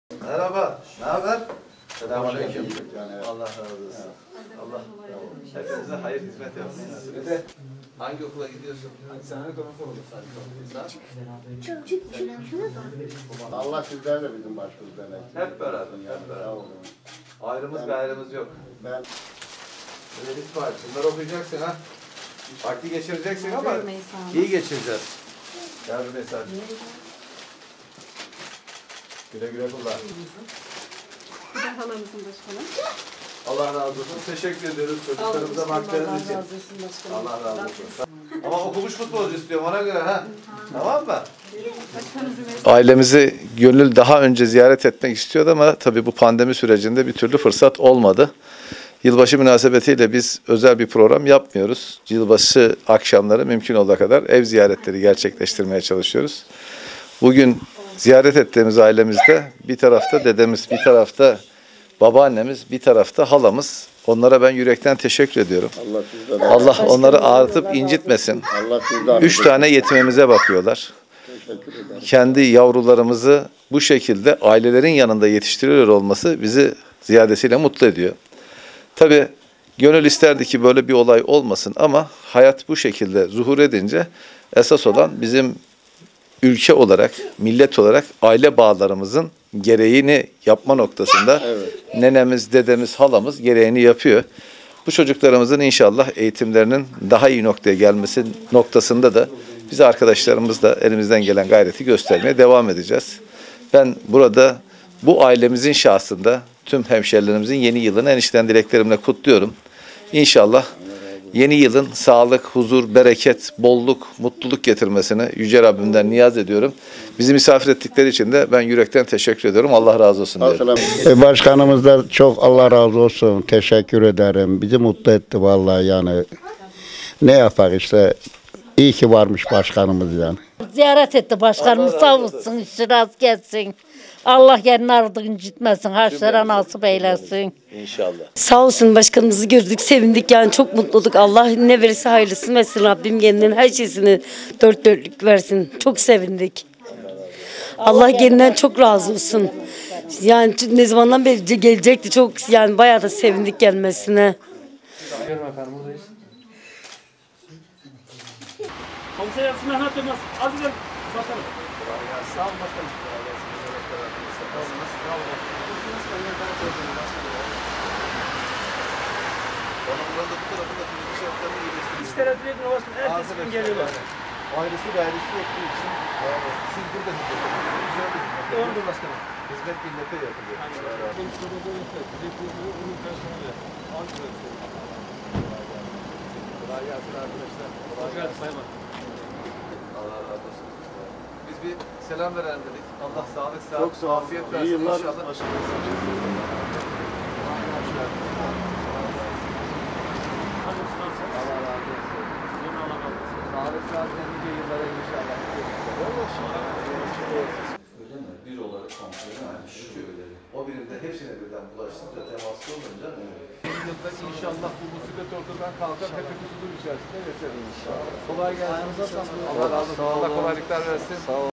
FADILOĞLU, YETİM ÇOCUKLAR İLE POLİSLERİ ZİYARET ETTİ
Şehitkamil Belediye Başkanı Rıdvan Fadıloğlu, yeni yıla dakikalar kala yetim çocuklu ailelerin ve polislerin misafiri oldu. Yeni yılın hayırlara vesile olmasını temenni eden Başkan Fadıloğlu, "Yeni yılın sağlık, huzur, bereket, bolluk, mutluluk getirmesini yüce Rabbimden niyaz ediyorum" dedi.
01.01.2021